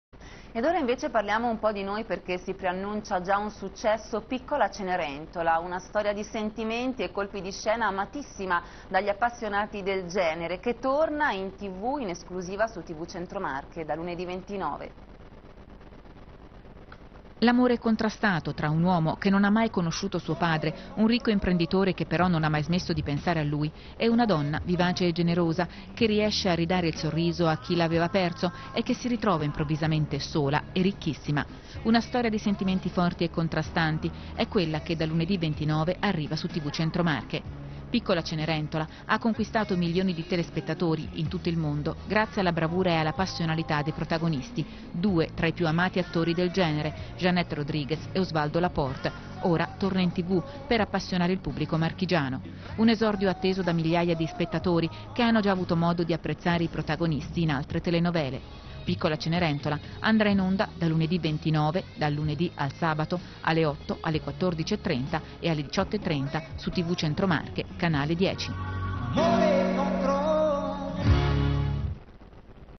Piccola Cenerentola - Servizio TG Tv Centro Marche
Servizio del telegiornale di TV Centro Marche dedicato alla telenovela Piccola Cenerentola